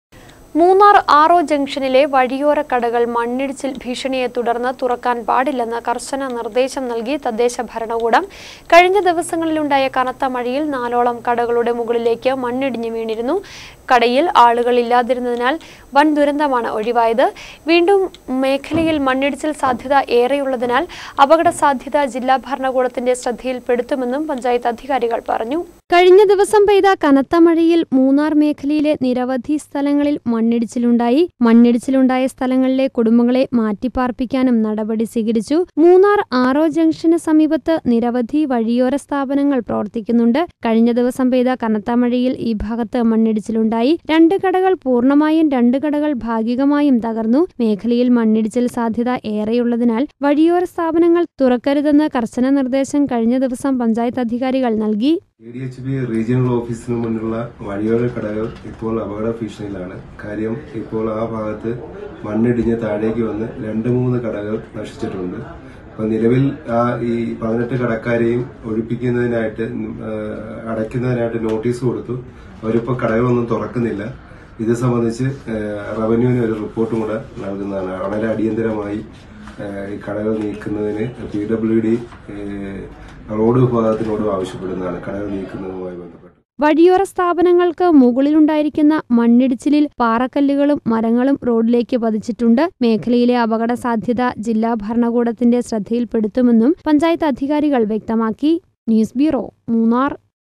മൂന്നാറിലെ വഴിയോരക്കടകള്‍ മണ്ണിടിച്ചില്‍ ഭീഷണിയെ തുടര്‍ന്ന് തുറക്കാന്‍ പാടില്ലെന്ന് നിര്‍ദ്ദേശം
മൂന്നാര്‍ ആര്‍.ഒ ജംഗ്ഷനിലെ വഴിയോരക്കടകള്‍ മണ്ണിടിച്ചില്‍ ഭീഷണിയെ തുടര്‍ന്ന് തുറക്കാന്‍ പാടില്ലെന്ന് കര്‍ശന നിര്‍ദ്ദേശം നല്‍കി തദ്ദേശ ഭരണകൂടം. കഴിഞ്ഞ ദിവസങ്ങളില്‍  ഉണ്ടായ കനത്ത മഴയില്‍ നാലോളം കടകളുടെ മുകളിലേക്ക് മണ്ണിടിഞ്ഞു വീണിരുന്നു. കടയില്‍ ആളുകള്‍ ഇല്ലാതിരുന്നാല്‍ വന്‍ ദുരന്തം ഒഴിവായി. വീണ്ടും മേഖലയില്‍ മണ്ണിടിച്ചില്‍ സാധ്യത ഏറെ ഉള്ളതിനാല്‍ അപകട സാധ്യത ജില്ലാ ഭരണകൂടത്തിന്റെ ശ്രദ്ധയില്‍പ്പെടുത്തുമെന്നും പഞ്ചായത്ത് അധികാരികള്‍.